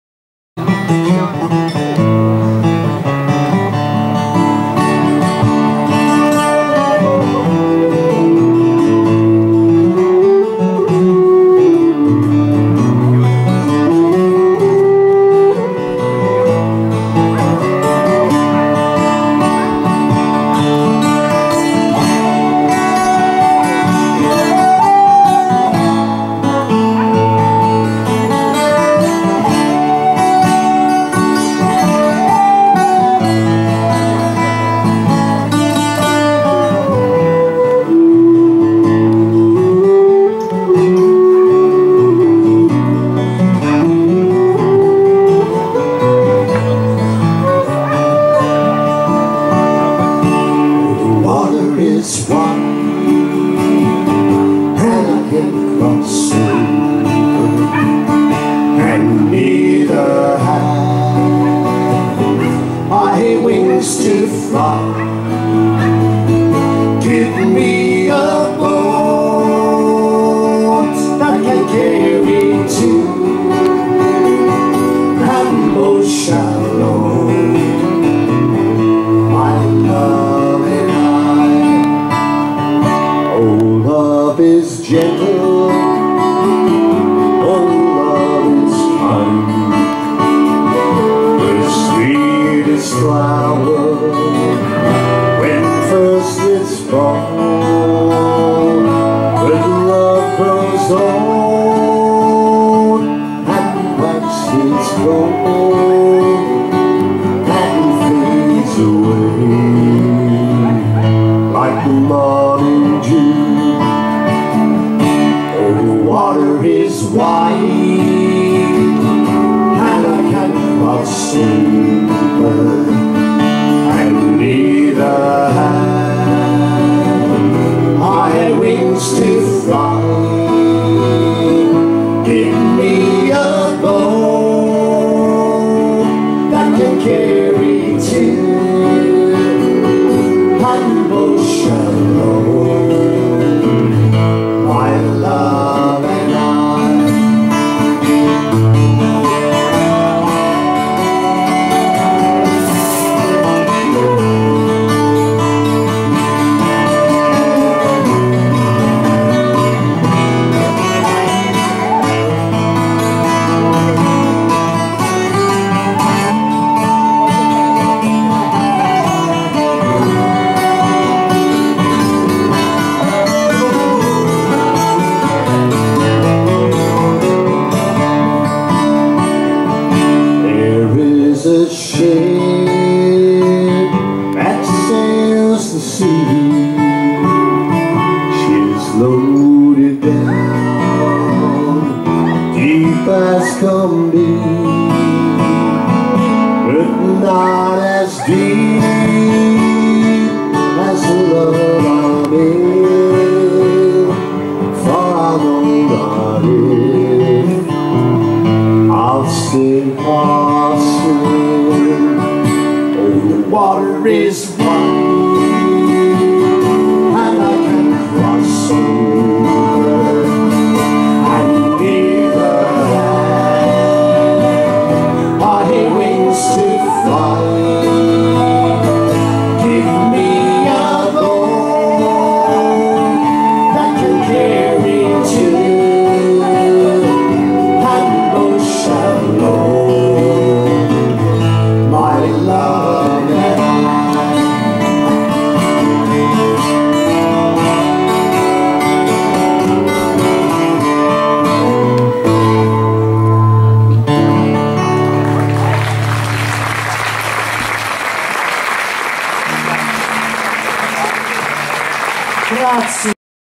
Matching Ties - parte 3 - Arcipelaghi Sonori - San Cesario sul Panaro
Giovedì 18 agosto 2016, parco di Villa Boschetti. 14a edizione di Arcipelaghi Sonori, festival di musica etnica a San Cesario sul Panaro (MO). MATCHING TIES: Irish, British, American-Celtic Folk.